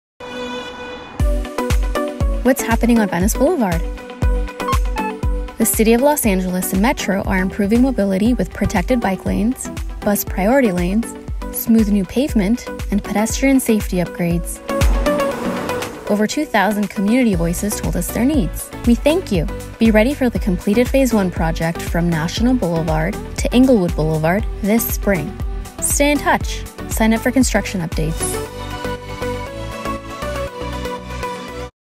Venice Blvd Safety and Mobility Improvements
LADOT is near completion of delivering safety and mobility improvements including protected bike lanes and bus lanes to Venice Blvd!